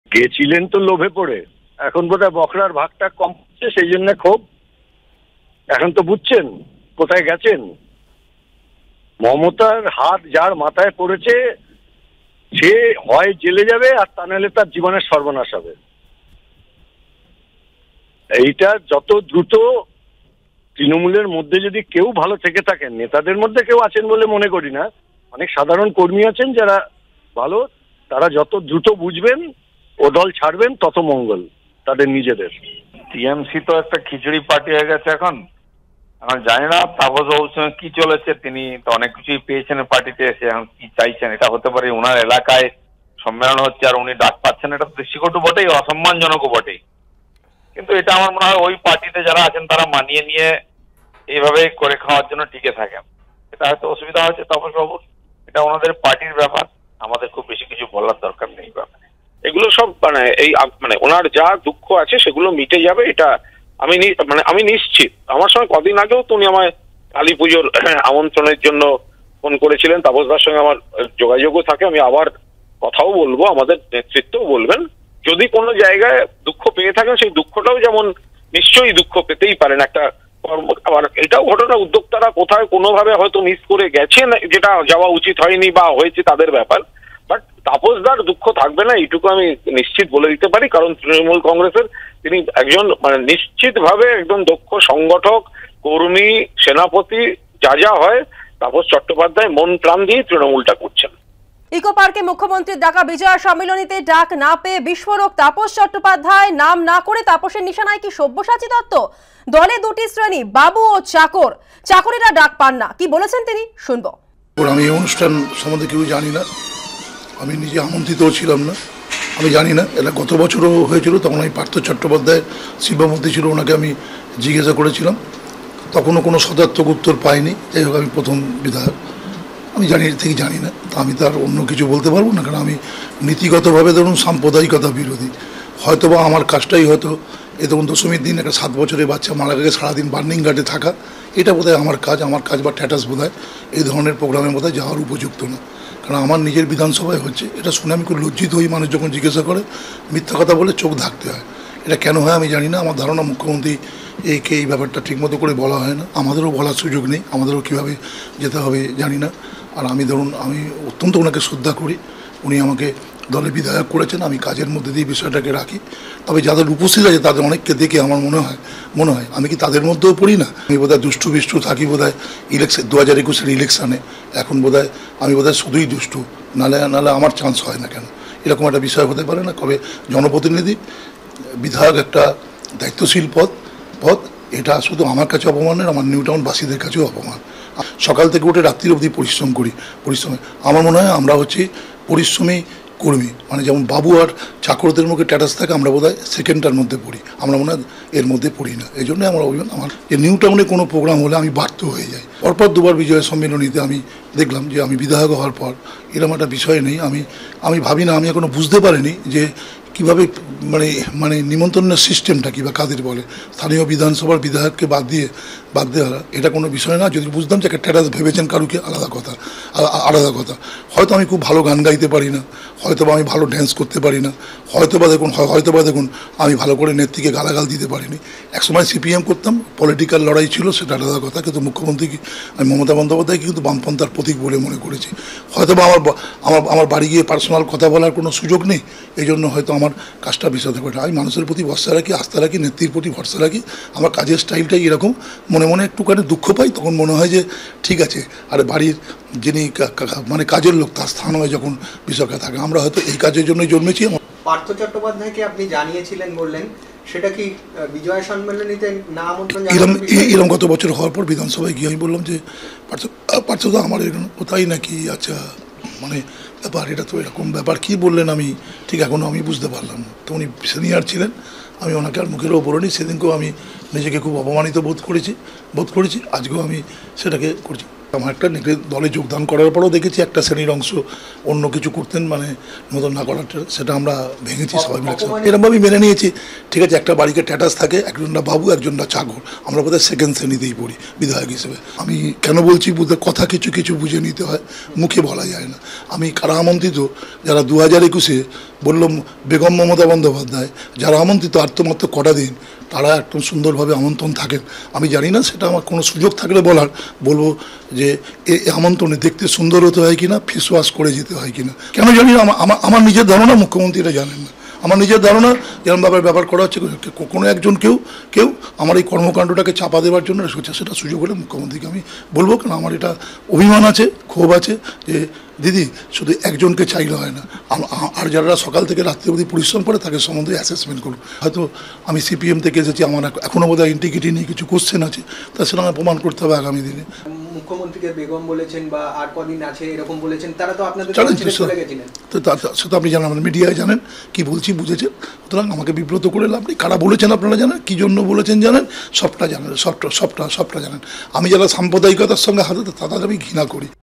Samik Lahiri: 'লোভে পড়ে তৃণমূলে যোগ দিয়েছিলেন তাপস চট্টোপাধ্যায়', আক্রমণ শমীক লাহিড়ির
'লোভে পড়ে তৃণমূলে যোগ দিয়েছিলেন তাপস চট্টোপাধ্যায়', আক্রমণ শমীক লাহিড়ির